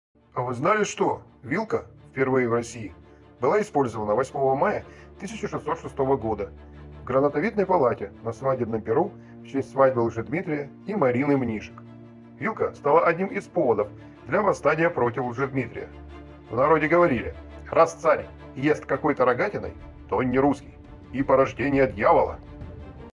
Интересные факты из истории... 43
А вы знали что вилка впервые в России  была использована 8 мая в грановитой палате на свадебном пиру в честь свадьбы Лжедмитрия и Марины мнишек. Вилка стала одним из поводом для восстания против Лжедмитрия в народе говорили-раз царь есть какой то рогатиной, то он не русский и порождение дьявола.